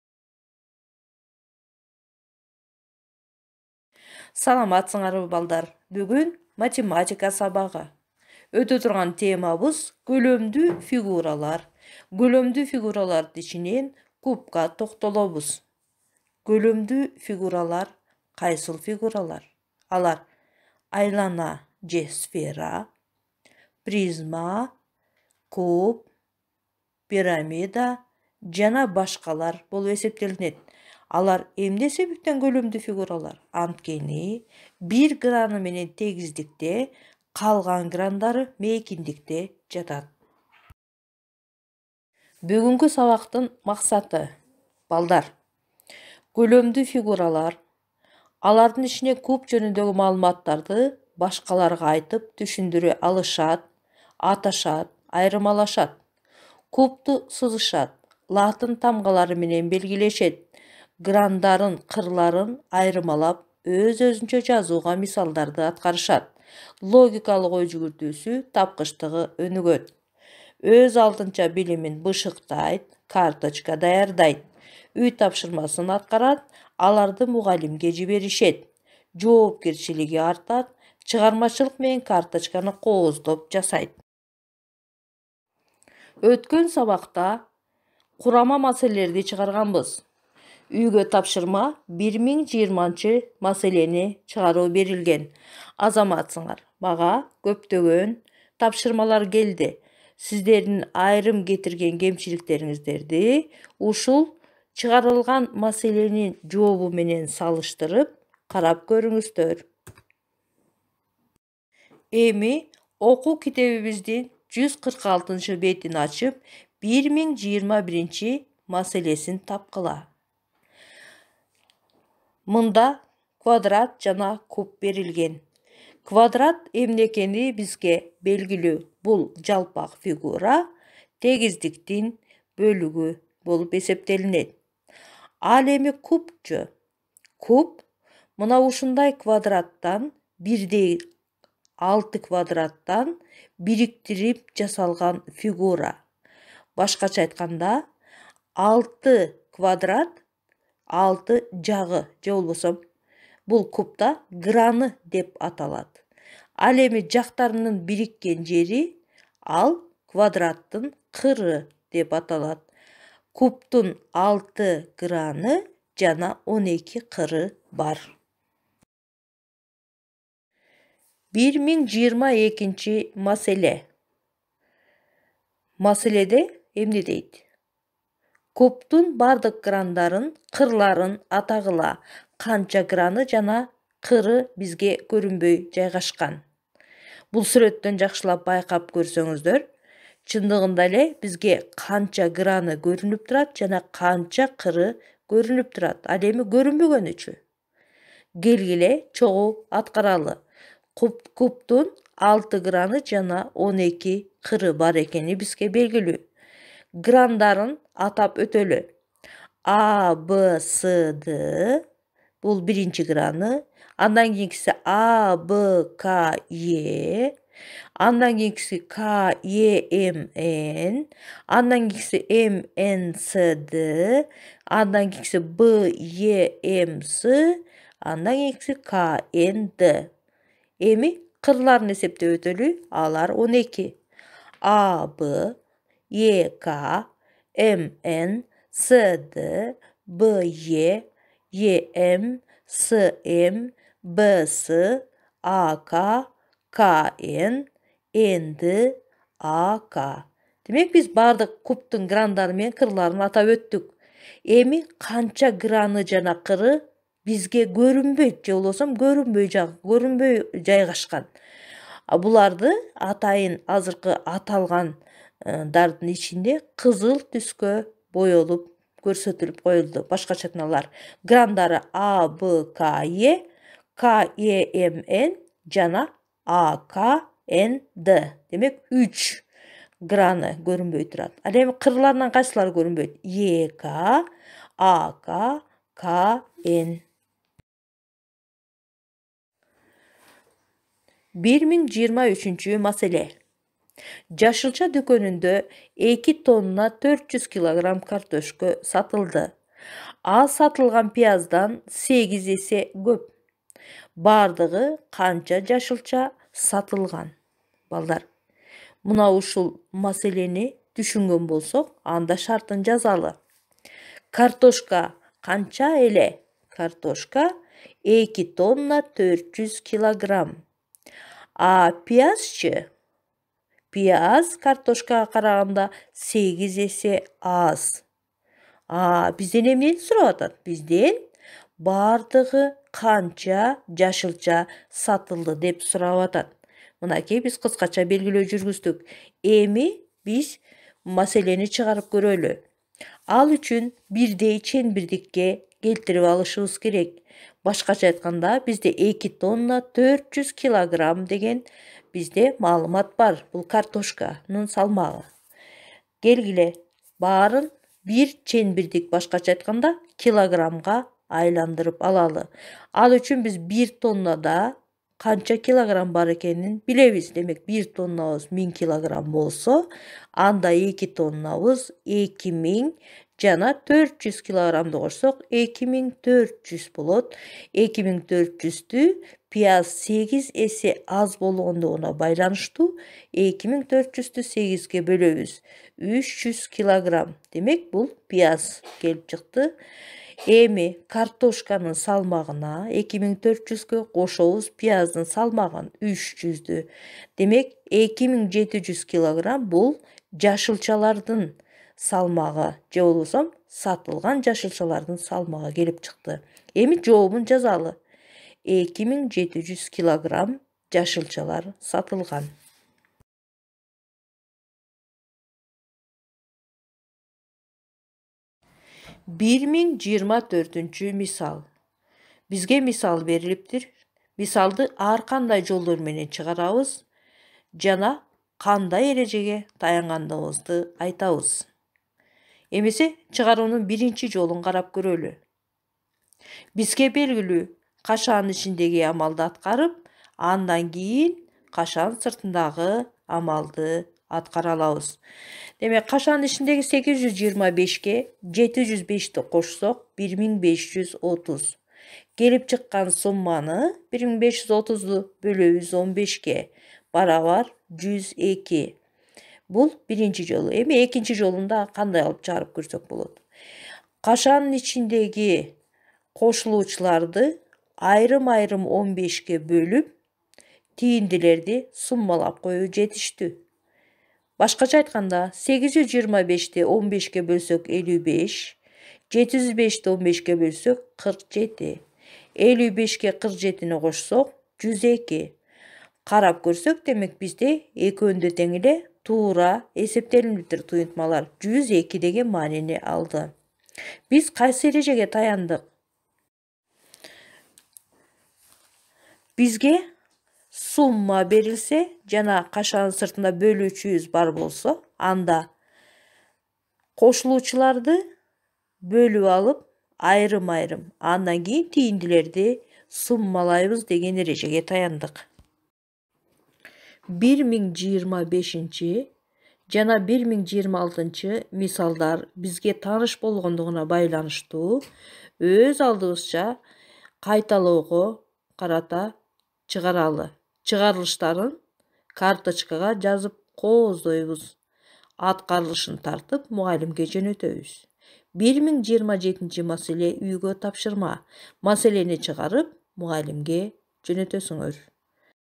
Көлөмдүү фигуралар.
Окуучулар көлөмдүү фигуралар түшүнүктөрүнө ээ болот, алардын ичинен кубдун грандыры, кырлары туралуу маалымат алышат жана аларга карата мисалдарды аткарышат.